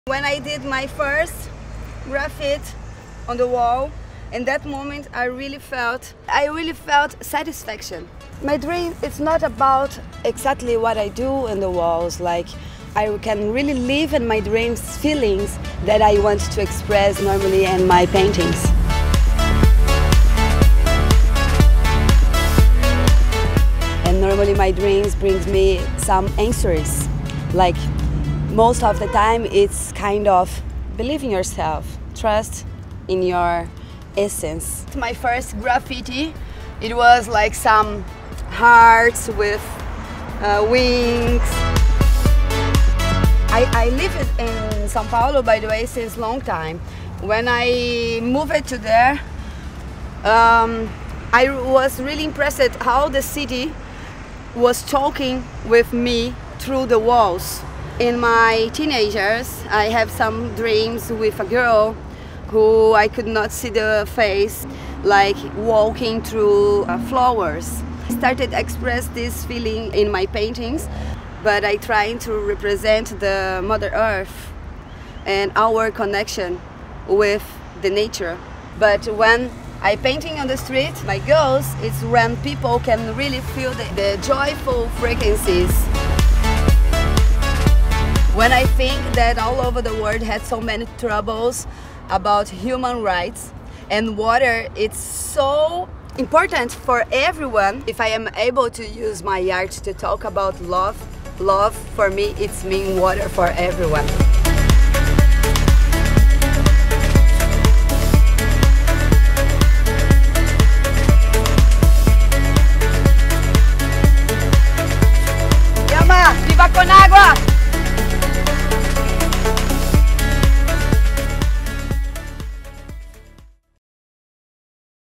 Street Artist Profile : Mari Pavanelli -- with Viva con Aqua
Painted in Cape Town, raising awareness on water for the launch on Viva con Aqua in South Africa.